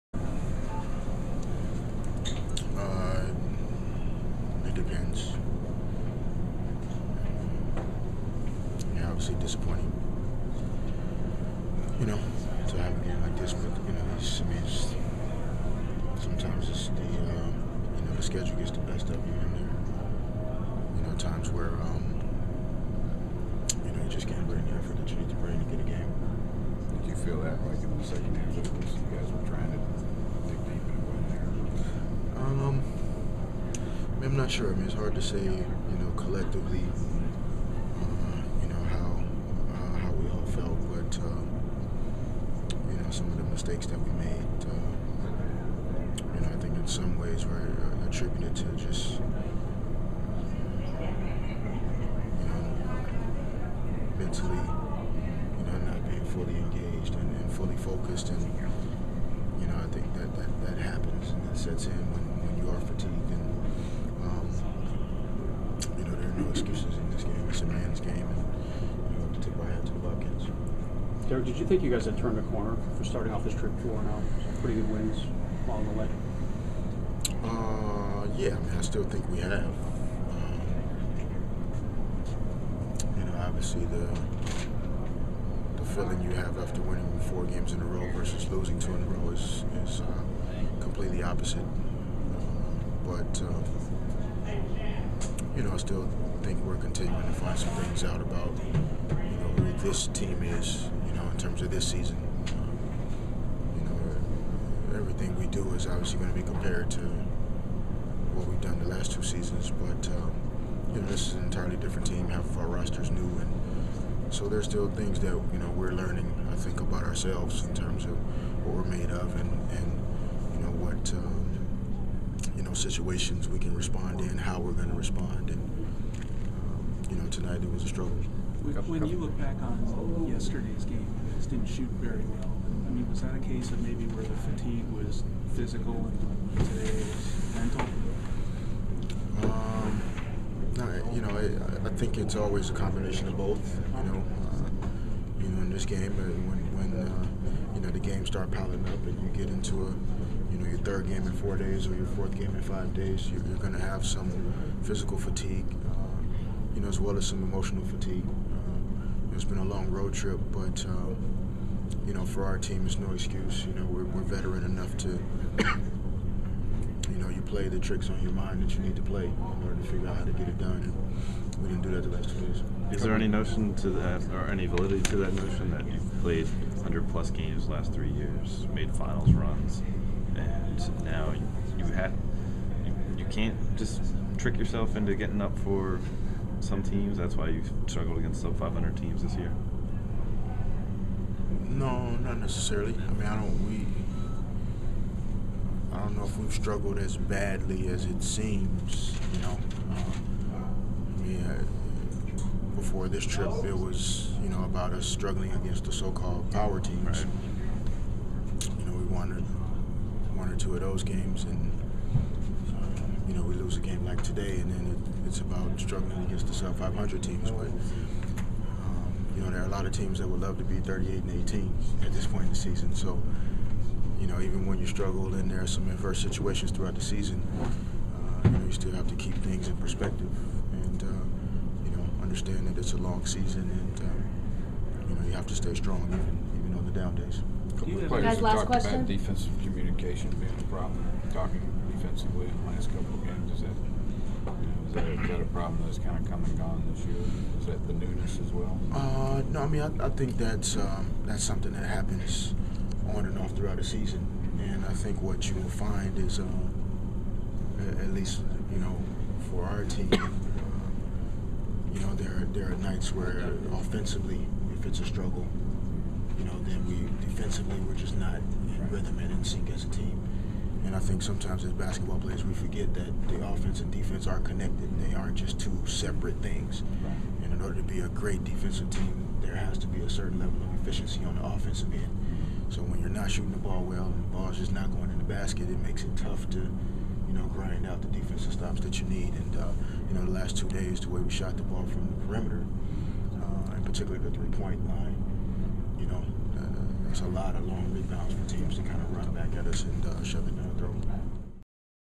Derek Fisher Post Game vs Charlotte.mov
LA Laker Derek Fisher Talks post game after the Lakers 109-89 loss to the Charlotte Bobcats